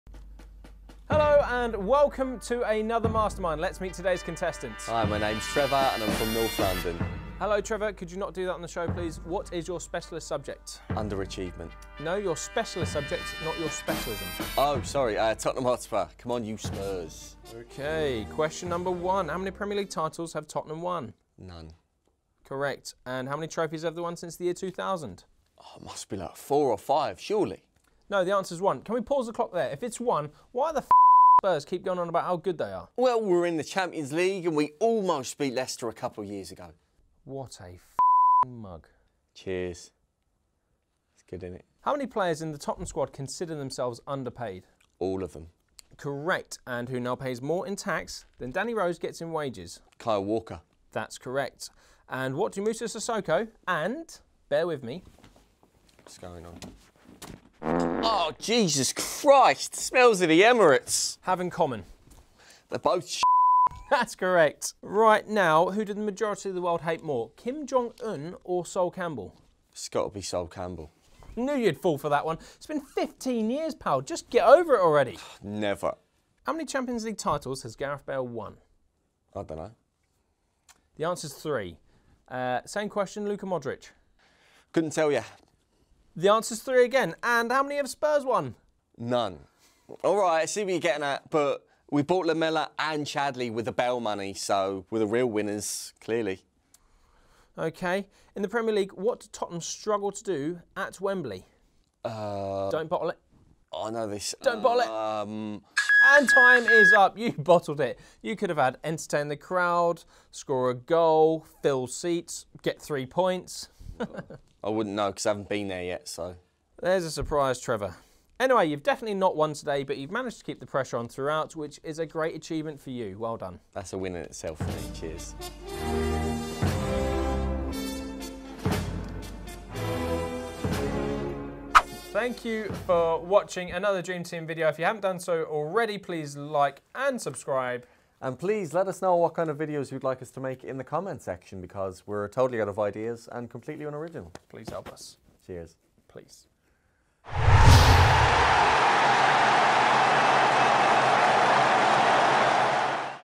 MASTERMIND - SPURS EDITION - EPISODE 4
In the Mastermind studio this week, a Tottenham fan joins us.